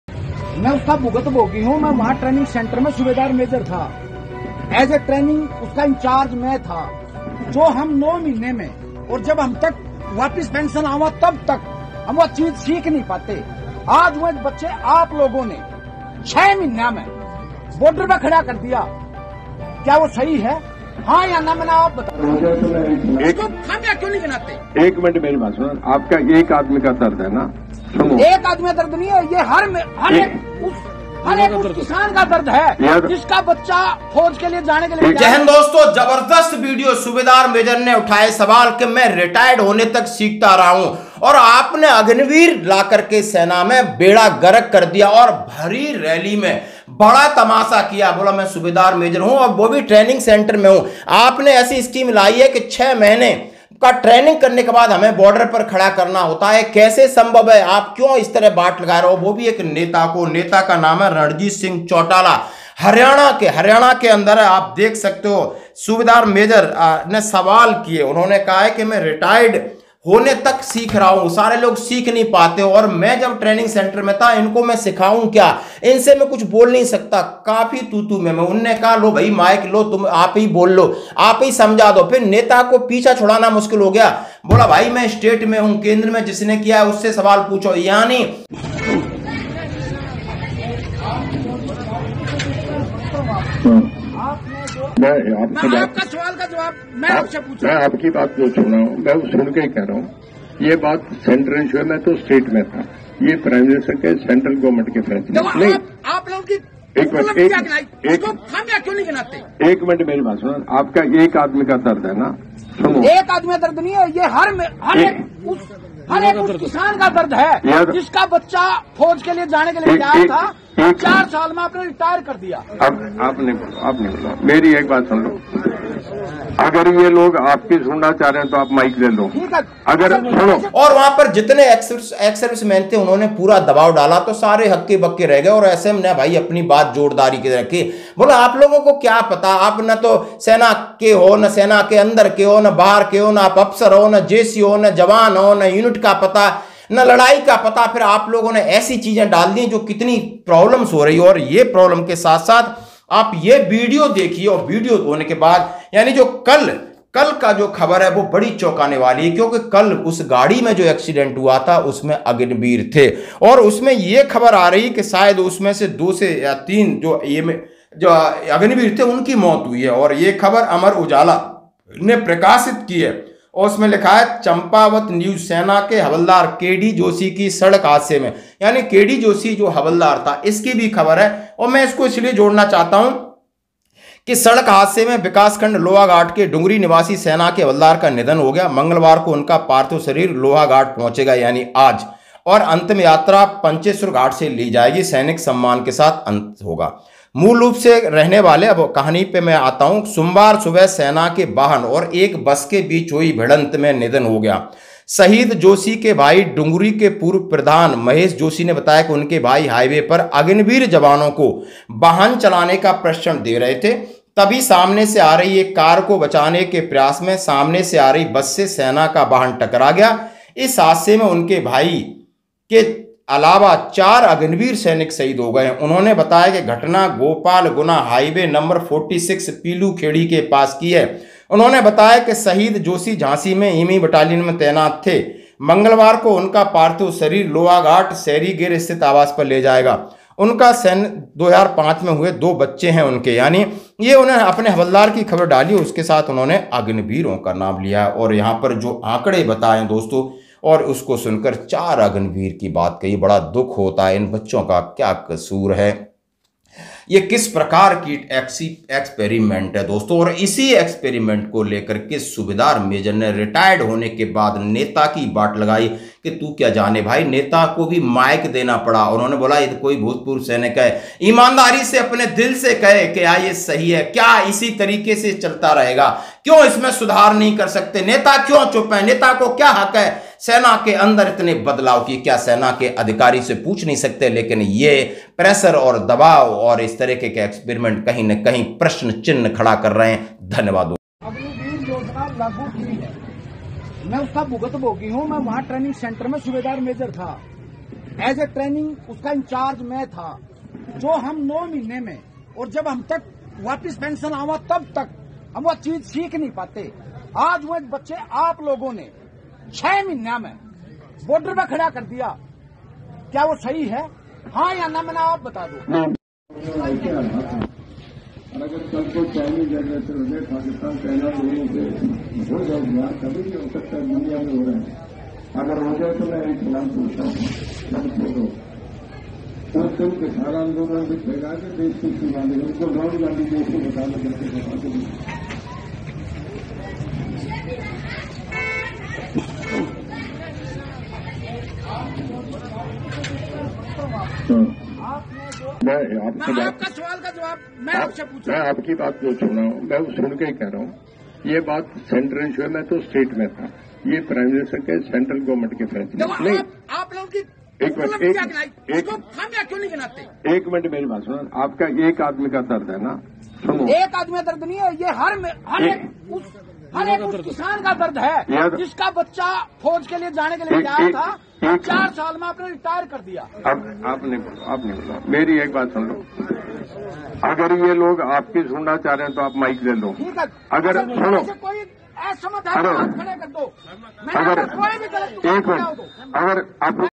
वाह,sub मेजर नेता को लगा दी फटकार, ऐसी की तैसी कर दी, हवलदार सहित चार अग्निवीर की मौत,
वाह, मेजर नेता को लगा दी फटकार, सभी के सामने ऐसी की तैसी कर दी, हवलदार सहित चार अग्निवीर की मौत, देखें बड़ी खबरFollow the Sainik Jeevan channel on WhatsApp